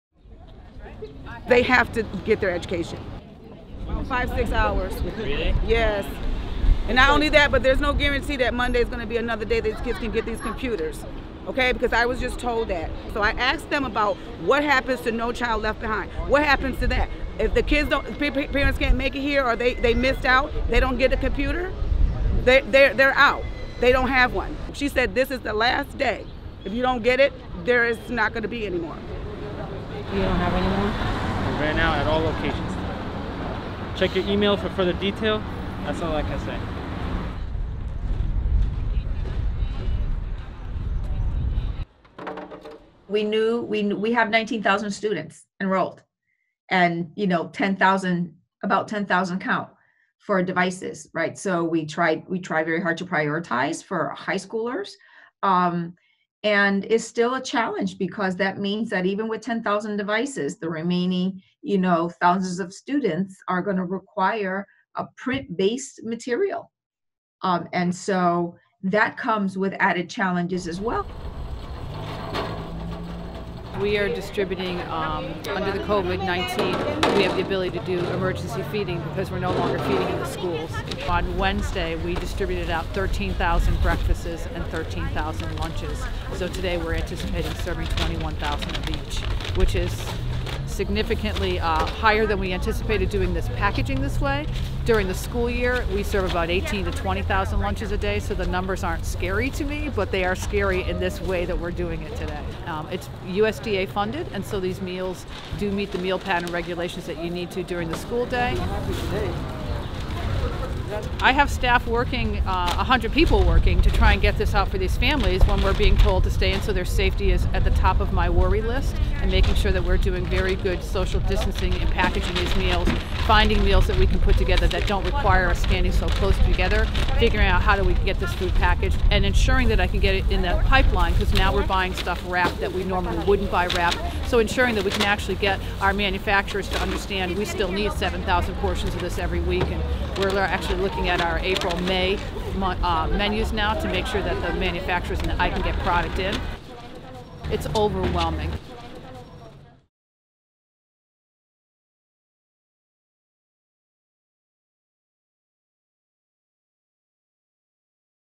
Hartford Public Schools Distribute Food And Laptops | CT Public
Families of school students waited in line for hours at Hartford’s Alfred E. Burr Elementary School this morning only to be told there were no more laptops. City schools start distance learning on Monday, March 30th through e-learning and tech-free learning. According to Superintendent Leslie Torres Rodriguez, educators are striving to meet the needs of families in their district, but they only have an estimated 10,000 devices for 19,000 students.